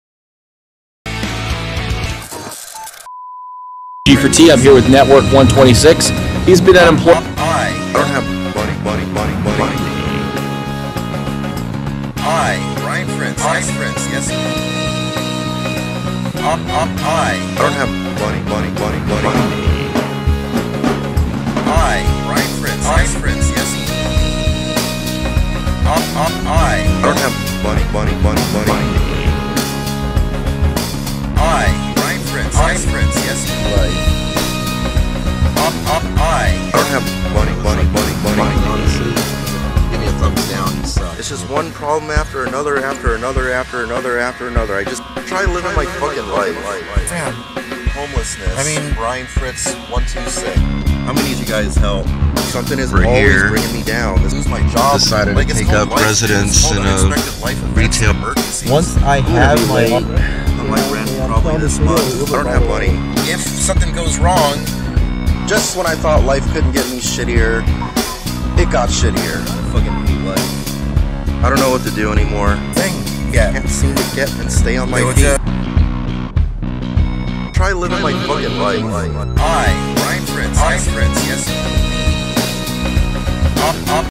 The Struggle Is Real (N32) Network126
Network126 try living his life. lol
A video inspired by YT maker LeftLong Beard (think its his track I snagged. just a fun wacky remix.